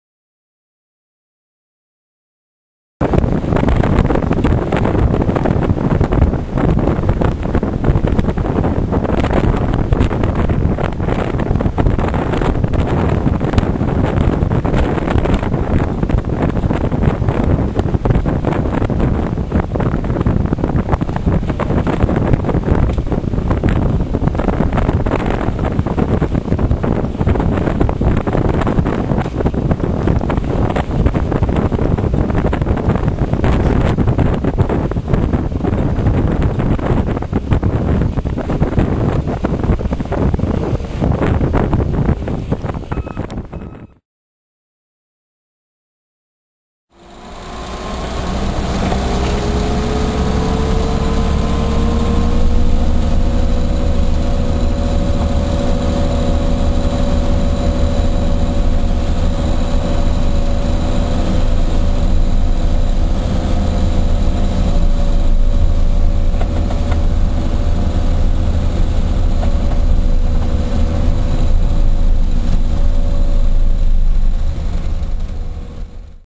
CAIRNGORM MOUNTAIN - JANUARY 2008 - PISTERS IN ACTION
KASSBOHRER ACTION ON THE CAS AND FIACAILL TRAVERSE